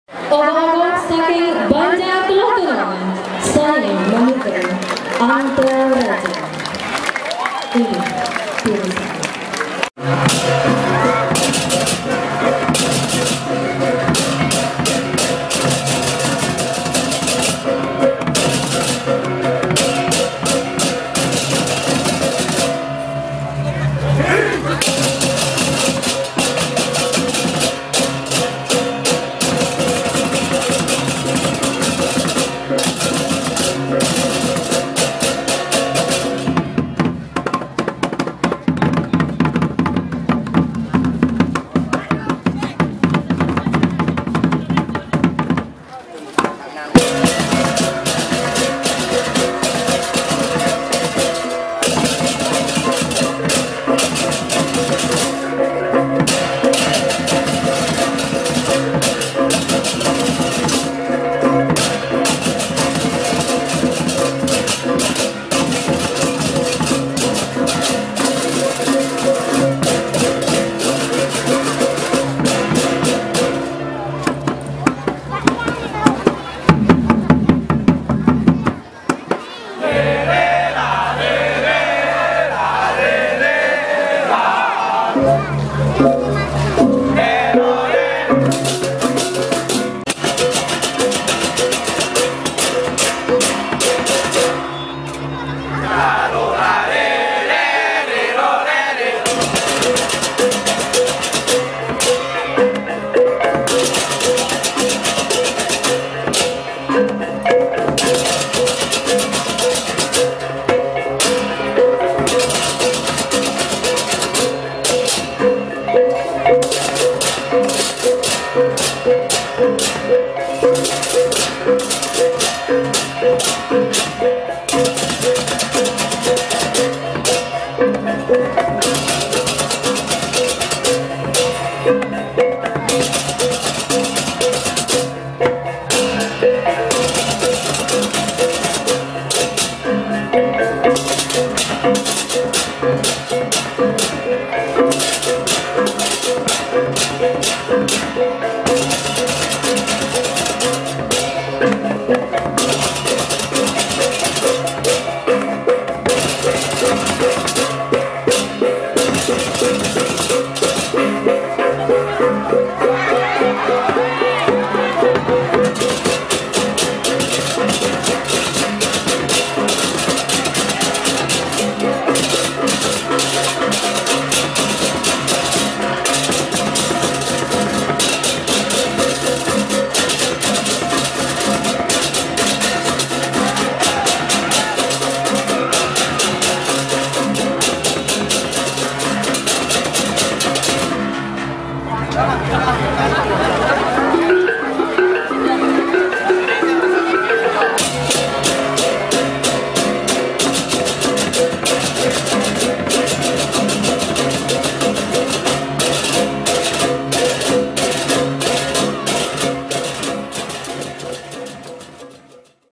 Parade Ogoh Ogoh Desa Batuan 2019 - Banjar Dlod Tunon
Pengrupukan diadakan pawai Ogoh Ogoh
Peserta dari Banjar Dlod Tunon Batuan